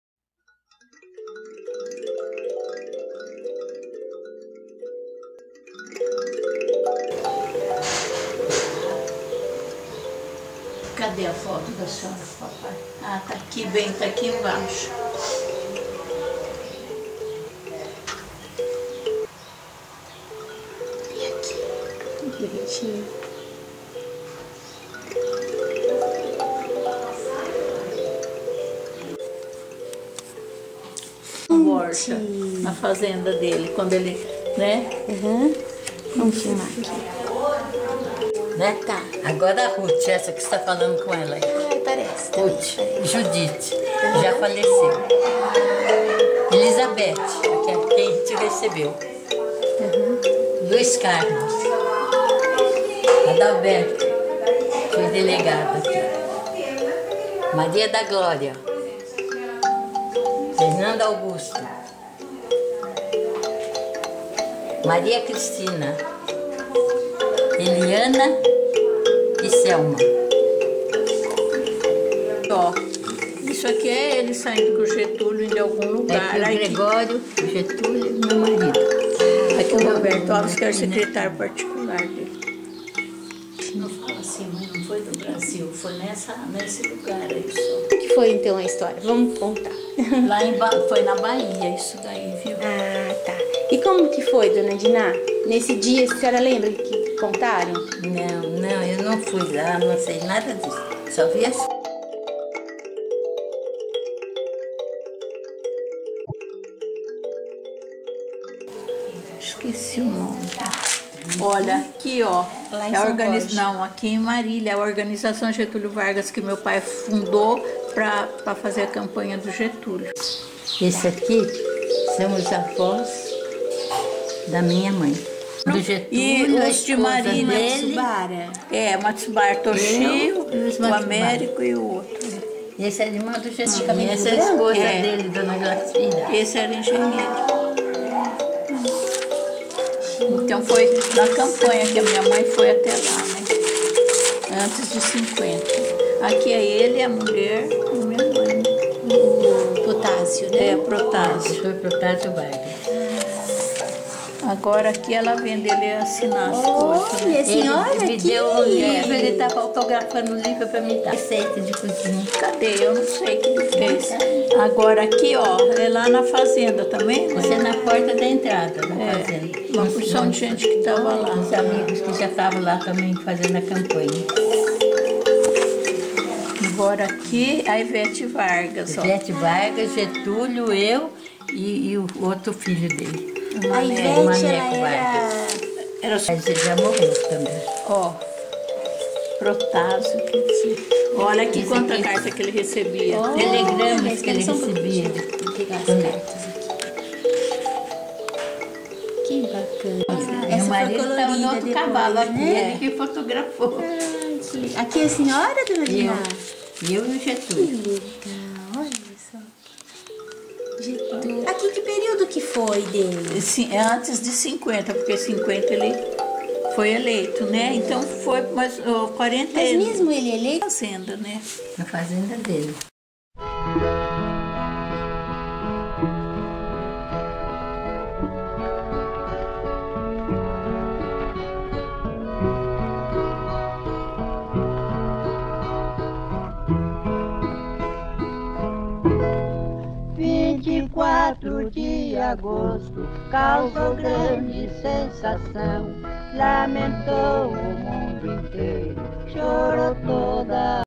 O amigo de Getúlio Vargas
Curta sobre a amizade de Getulio Vargas com a familia Manh
ães